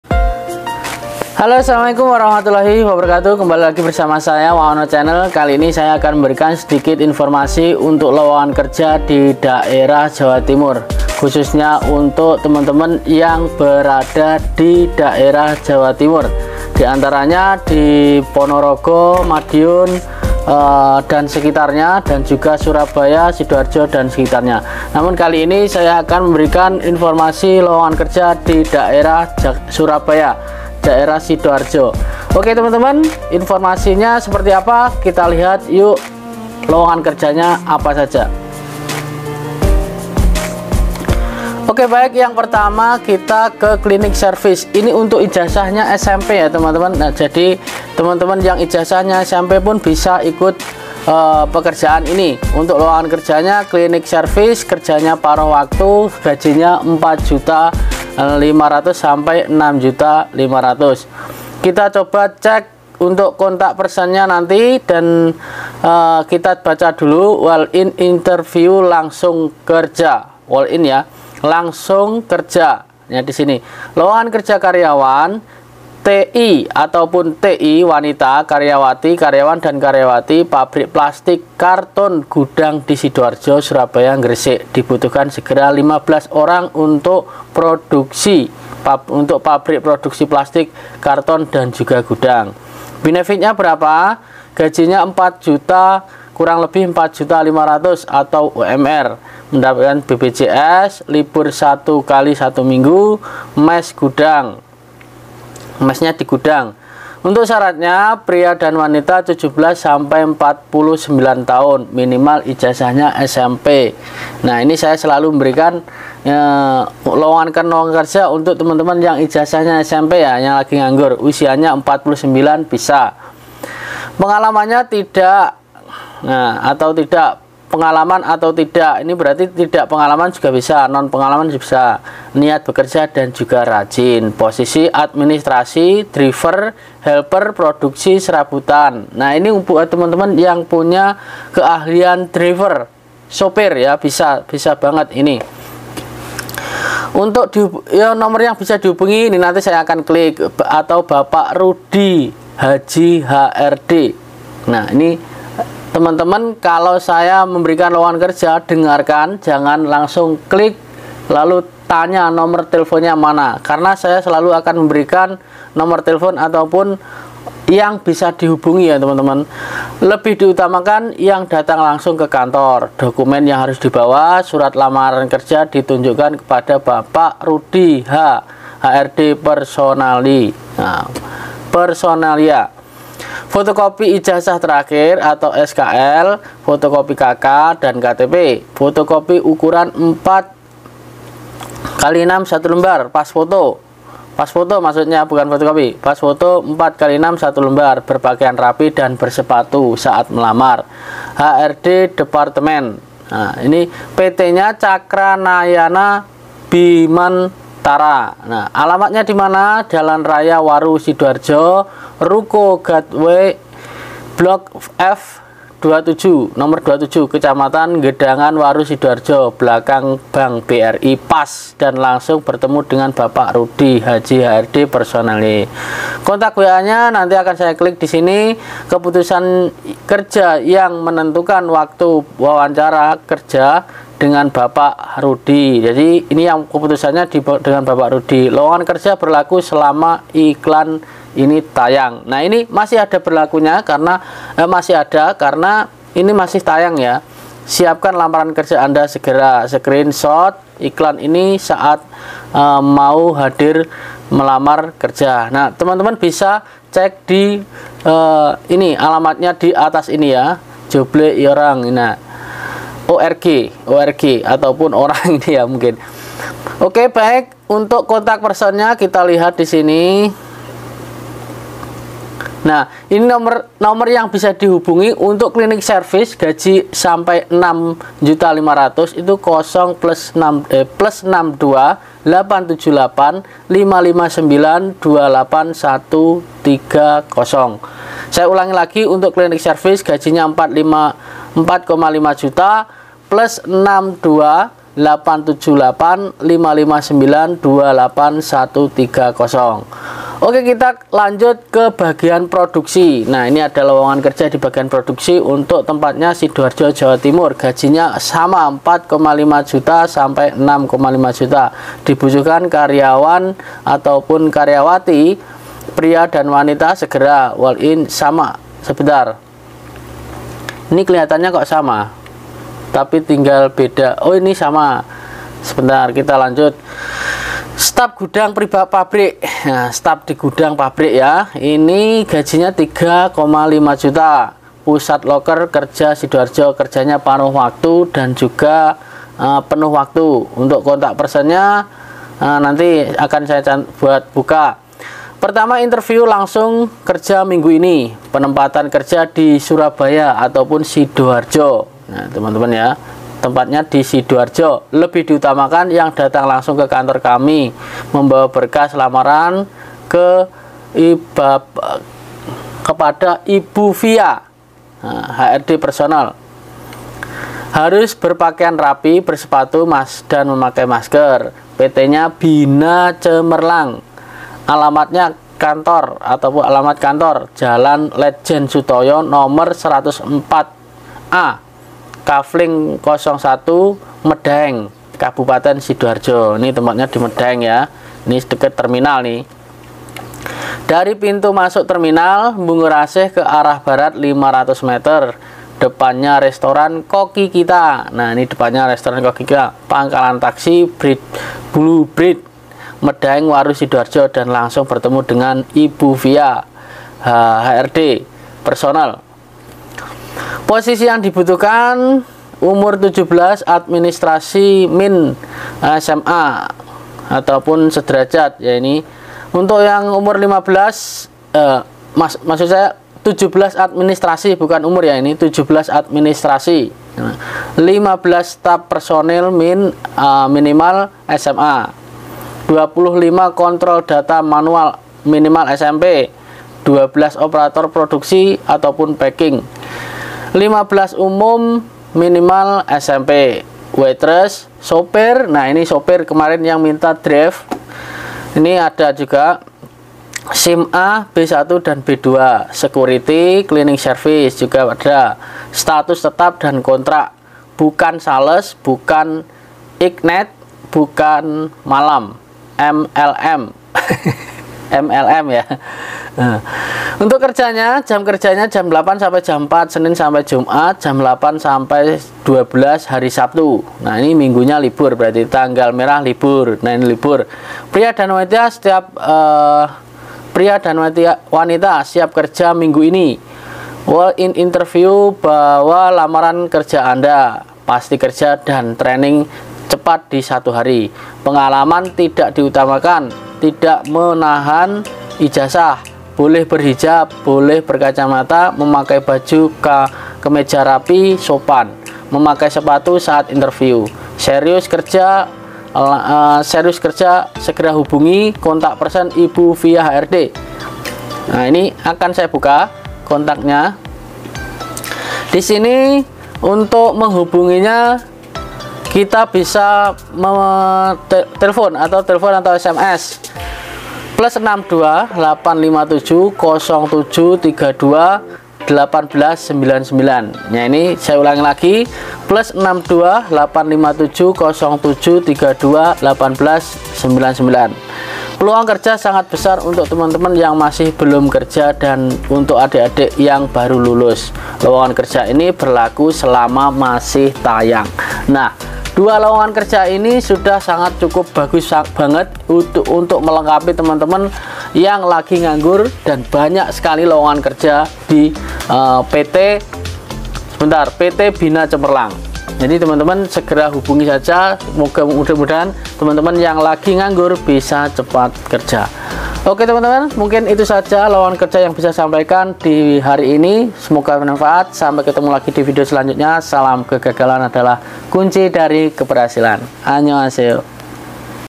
🔴LOWONGAN KERJA TERBARU 2023 IJAZAH SMP.GAJI HINGGA 6.5JUTA PER BULAN.
Tag Info Lowongan Kerja .

InfoLowonganKerjaMei2023
13 Komentar